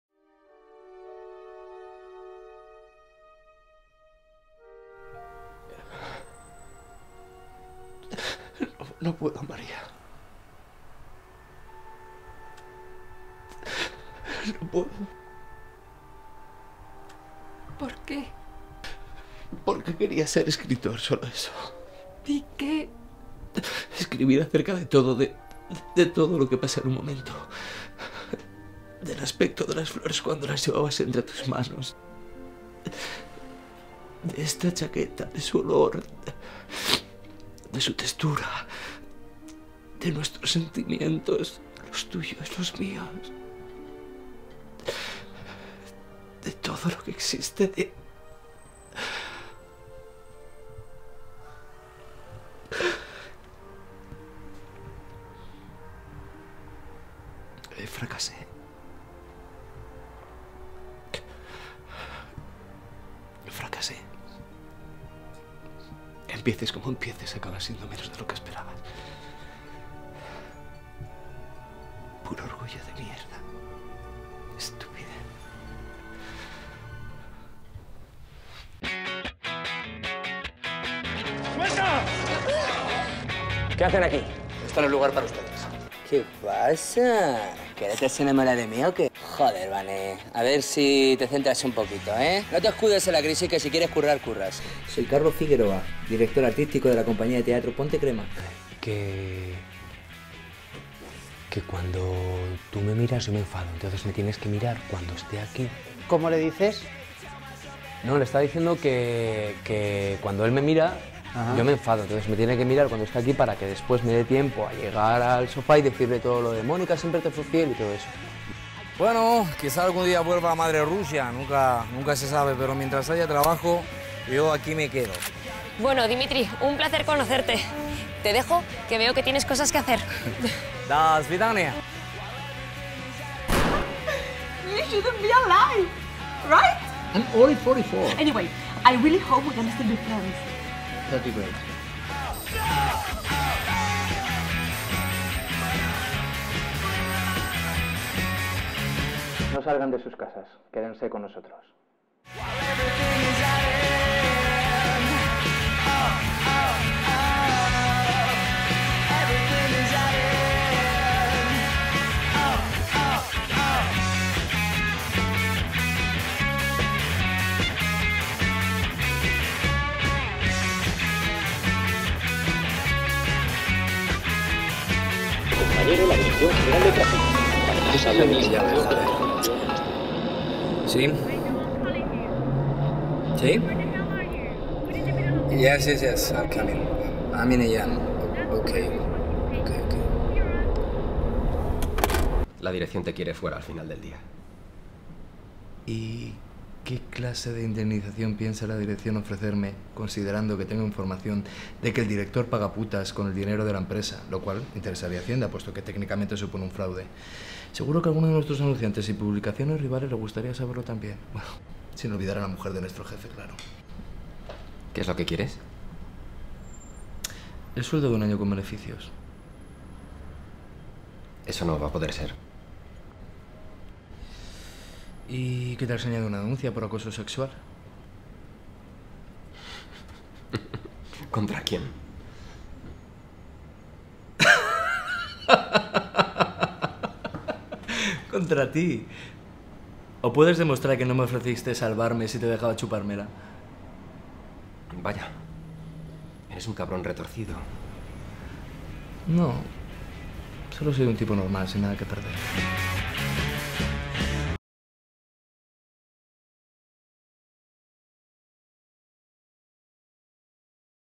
VIDEOBOOK2014 Rubén Martínez
Resumen de algunos trabajos audiovisulles realizados por el actor Rubén Martínez.